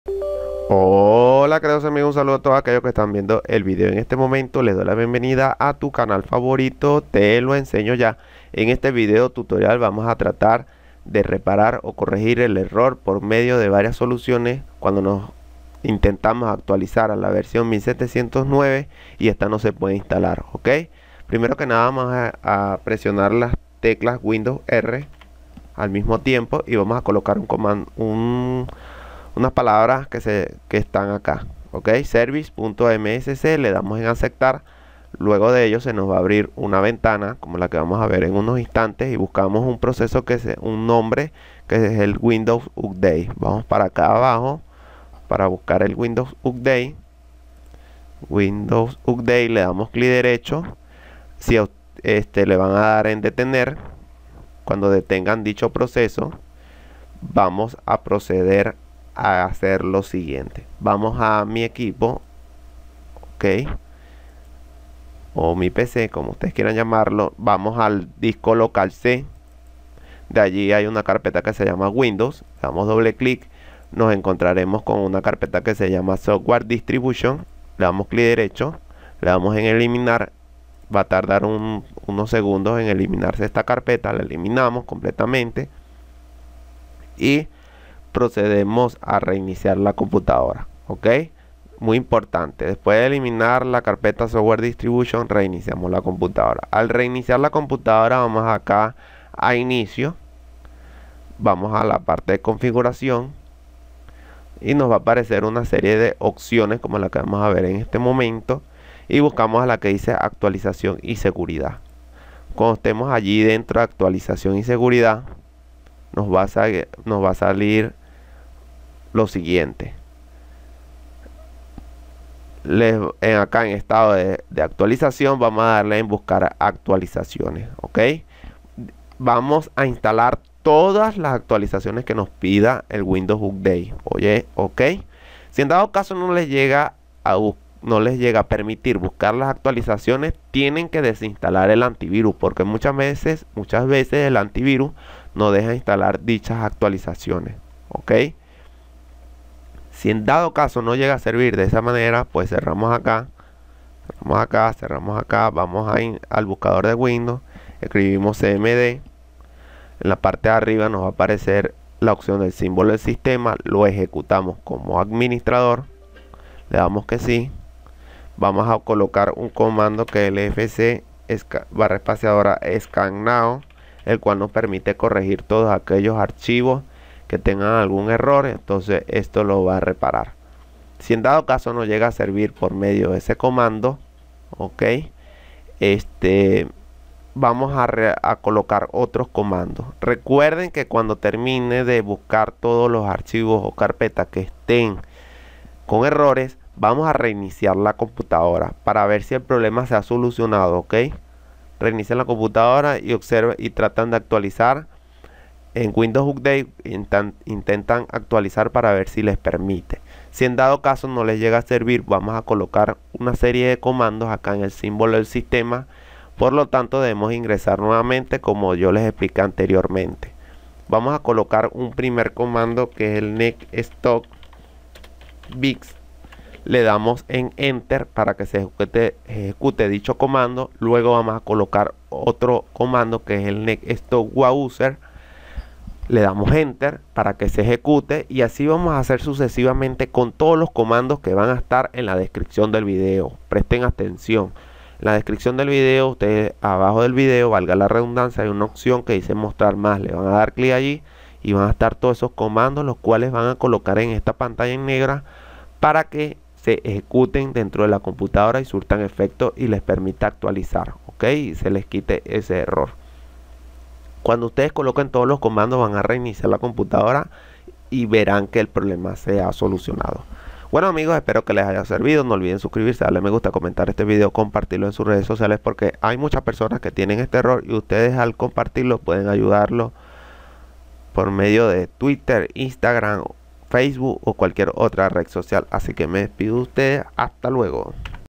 Error Actualización 1709 no se puede instalar en windows 10 I SOLUCIÓN 2024
En este video tutorial se dará la solución error windows 10 versión 1709 de una manera rápida y efectiva

  exit

Videos Recomendados
💛 Como ACELERAR Celular. Haz esto!